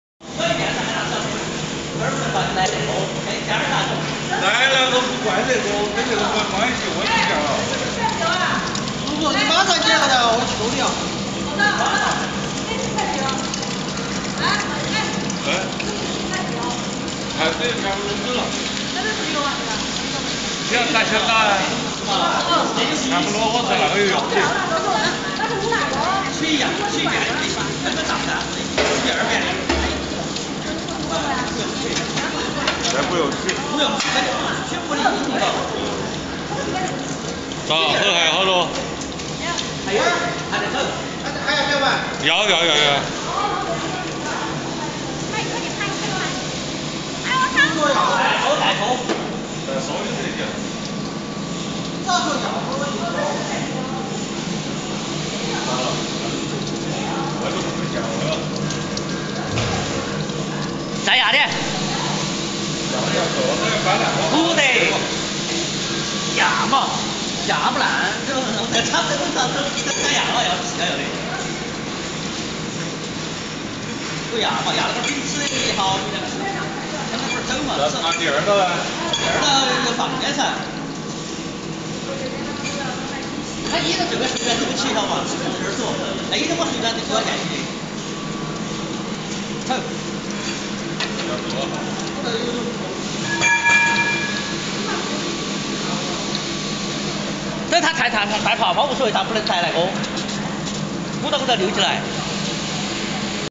Máy ép dầu, máy ép dầu lạc, máy ép dầu mè mới nhất LH
Máy ép dầu thực vật chuyên dùng ép dầu lạc, ép dầu mè, ép dầu đậu phộng cho cơ sở ép dầu thuê, hoặc sản xuất dầu ăn công nghiệp. Sản xuất dầu dừa
Video chạy thử ép dầu mè bằng công nghệ đốt nóng không cần ốp nhiệt giúp giảm chi phí sử dụng về lâu dài.